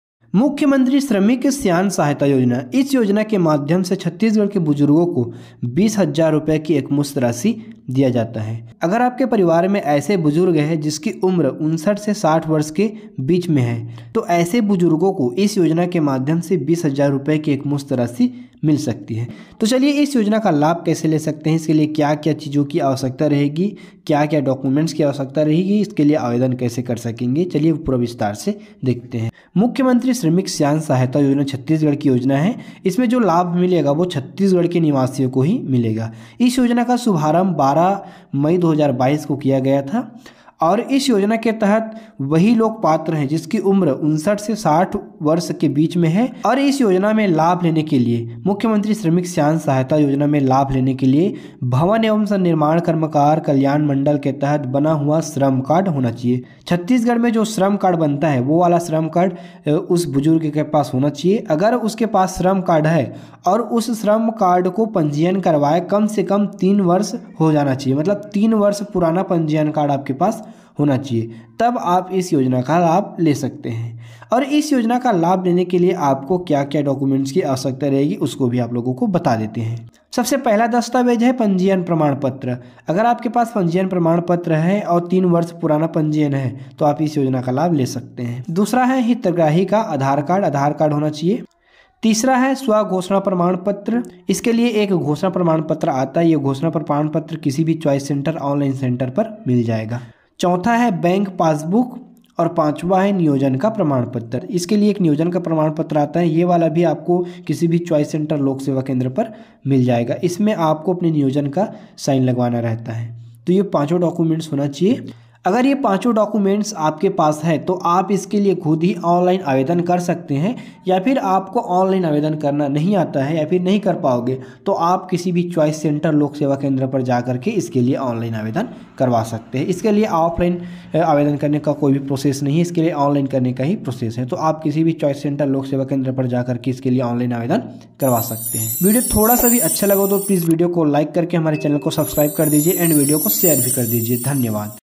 मुख्यमंत्री श्रमिक सियान सहायता योजना | Cg के बुजुर्गों को मिलेगी 20 हजार सलाना | Cg New Govt Scheme
Cg New Govt Scheme 2023 | Cg Govt देगी बुजुर्गों को 20 हजार सलाना | Cm श्रमिक सियान सहायता योजना

Cg New Govt Scheme 2023
Cg New Government Scheme

छत्तीसगढ़ भवन एवं अन्य सन्निर्माण कर्मकार मंडल के तहत मुख्यमंत्री श्रमिक सियान सहायता योजना चल रही है. इस योजना के तहत, पंजीकृत श्रमिकों को दुर्घटना के कारण इलाज कराने पर एकमुश्त राशि दी जाती है. यह राशि वास्तविक राशि या अधिकतम 20,000 रुपये हो सकती है. इसके अलावा, योजना के तहत ये सुविधाएं भी दी जाती हैं:
दृष्टिबाधित चश्मा के लिए 1,000 रुपये
कृत्रिम दांत के लिए 5,000 रुपये
श्रवण बाधित यंत्र के लिए 6,000 रुपये अस्पताल में 15 दिन से ज़्यादा रहने पर न्यूनतम राशि
11 सितंबर, 2023 को, श्रम विभाग ने जिले के दो पंजीकृत श्रमिकों को 40,000 रुपये की राशि उनके बैंक खाते में ट्रांसफ़र की थी. योजनाएं जगह और व्यक्तिगत परिस्थितियों के हिसाब से अलग-अलग हो सकती हैं.
Labour Department Chhattisgarh - छत्तीसगढ़ शासन श्रम विभाग